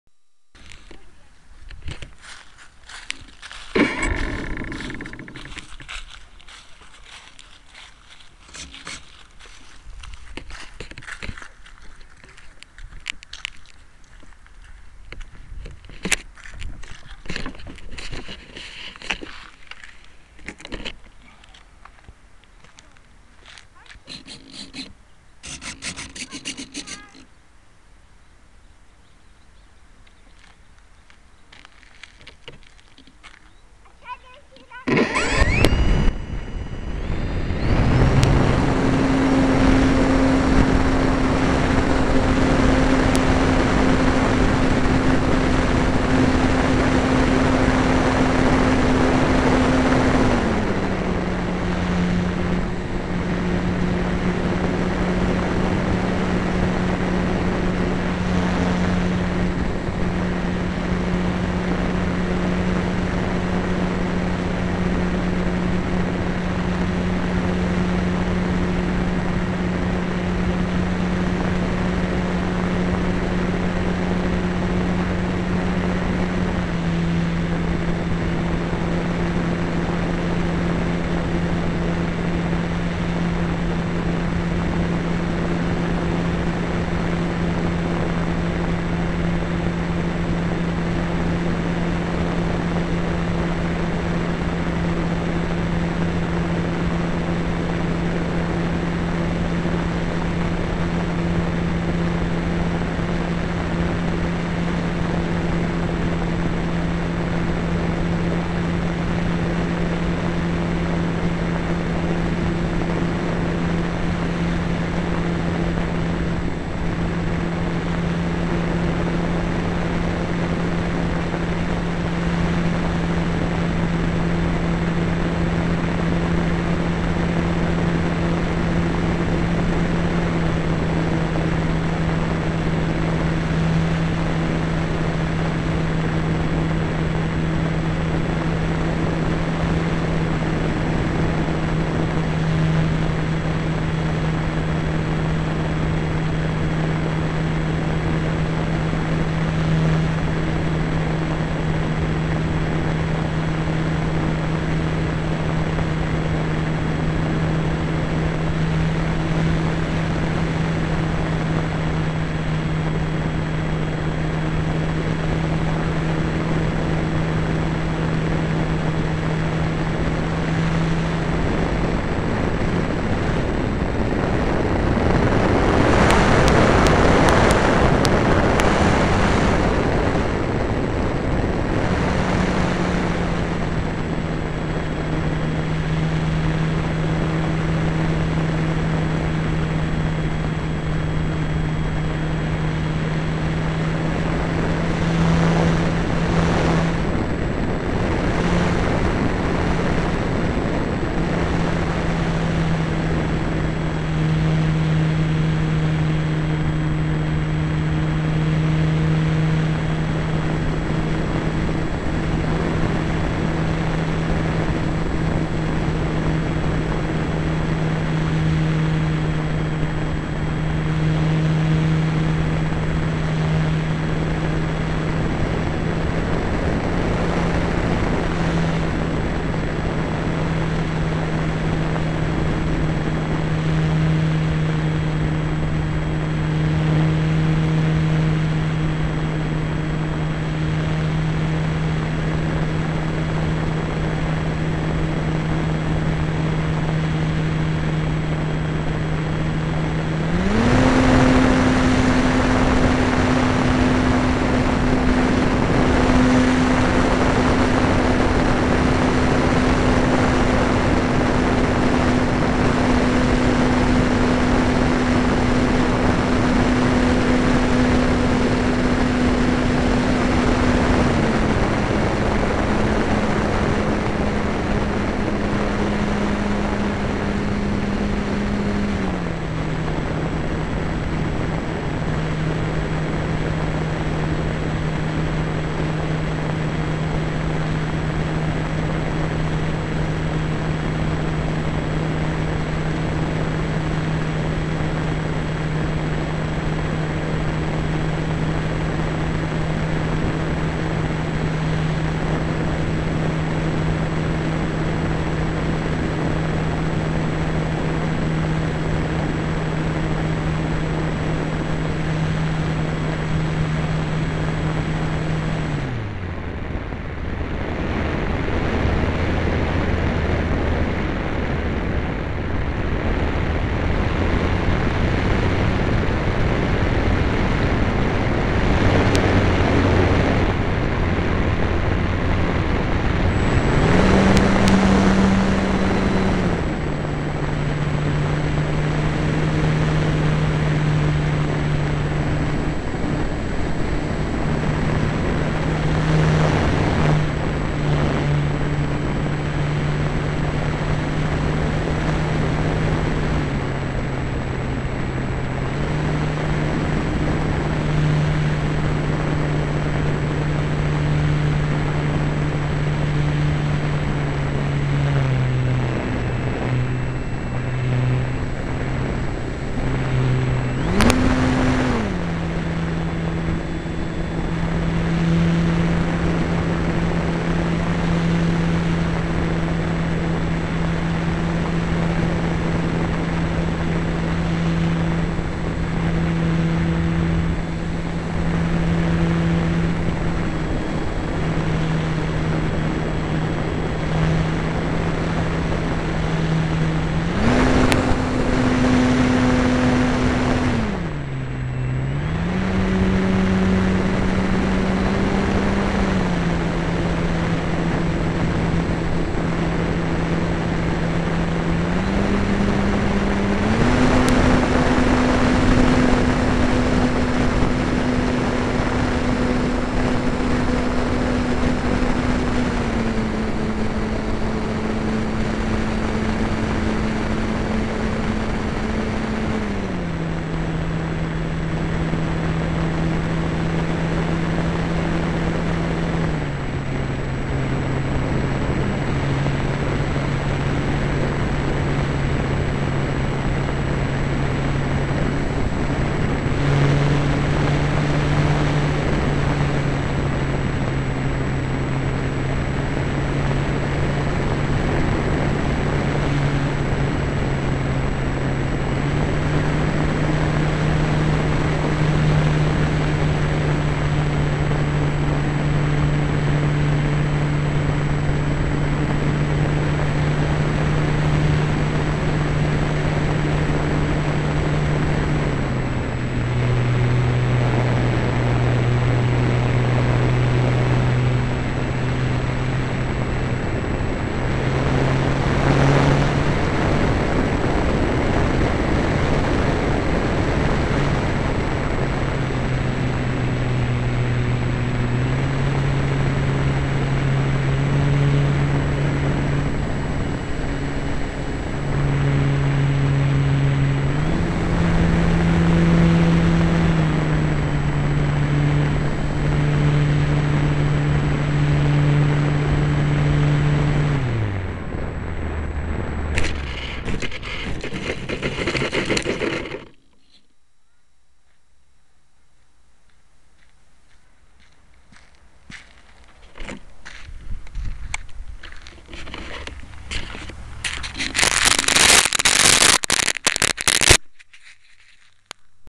cessna nad ihriskom UMB
jesenne polietanie s cessnou 480 nad sportovym arealom UMB ...